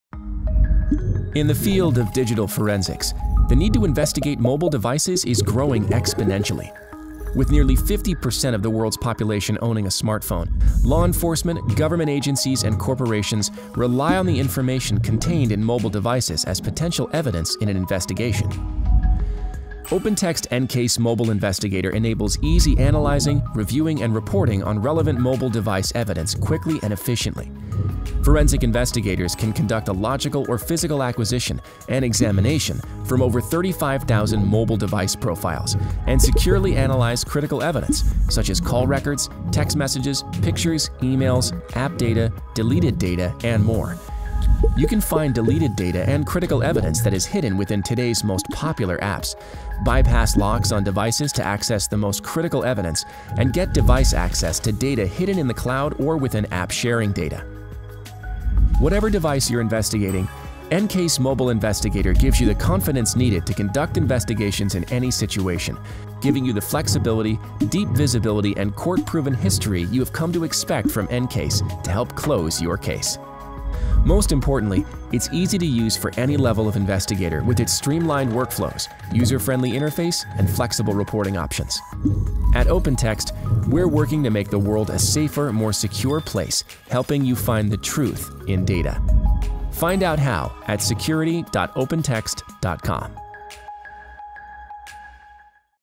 Learn how to conduct digital forensic investigations on mobile devices
With 67% of the world’s population owning a mobile device, evidence related to a criminal, civil or corporate investigation may be lurking in cell phones, watches and other mobile devices.  Watch this video to discover how to seamlessly conduct digital forensic investigations on a wide variety of mobile devices.

to learn how OpenText is helping you make the world a safer, more secure place by finding the truth in data.